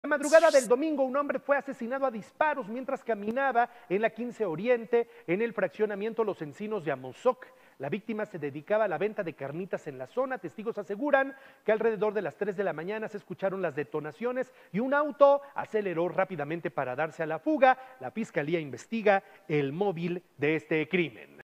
Ejecutan a hombre || Noticias con Juan Carlos Valerio
Fue ejecutado a disparos un hombre en Amozoc.